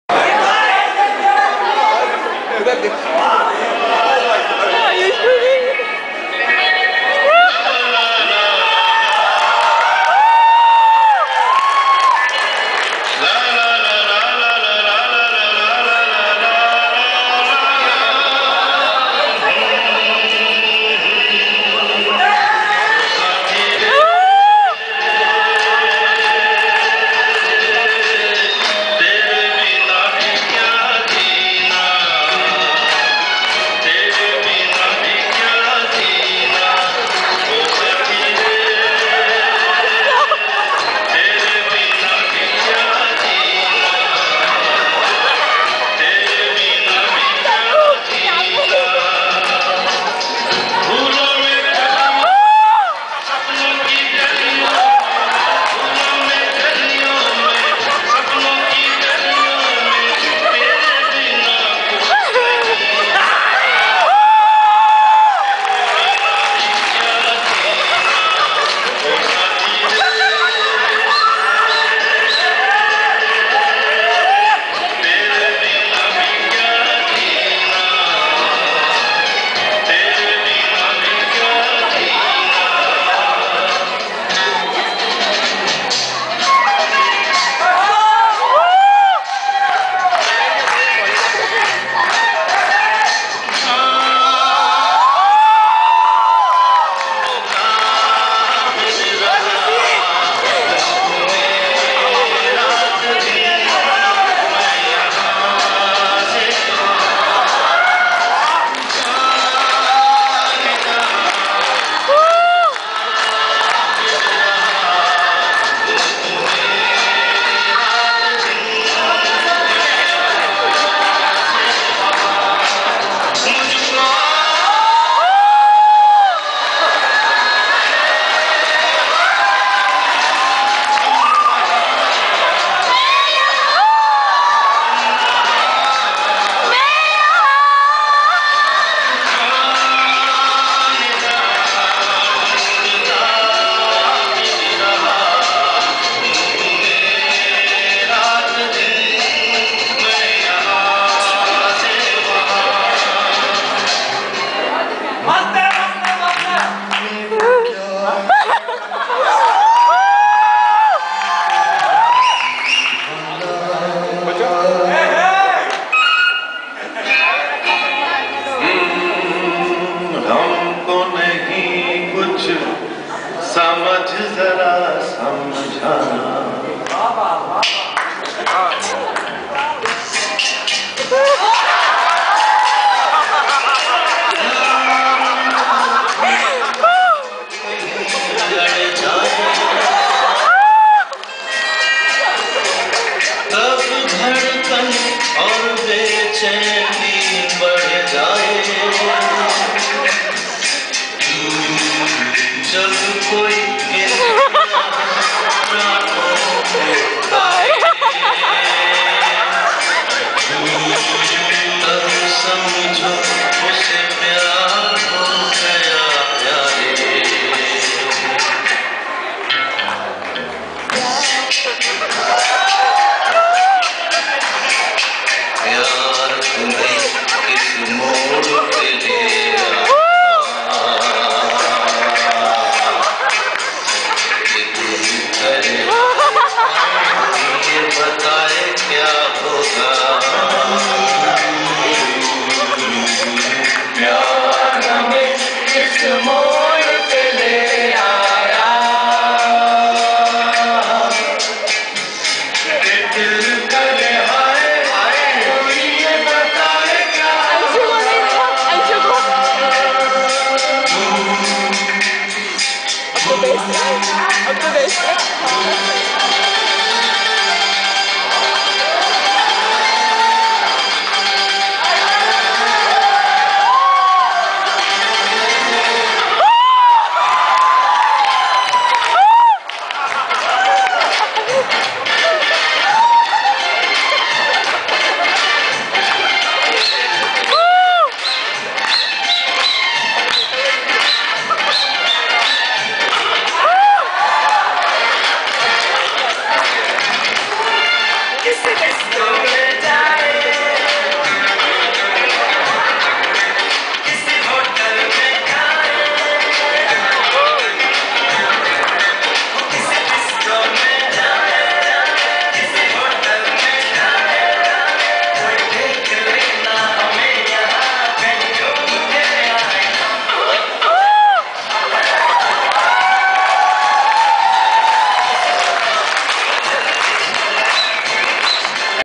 ULTIMATE boyz dance.MPG
RSH Shehnaai 2012 celebration
Performance by DOCTORATE BATCH 2011
National Institute of Immunology, India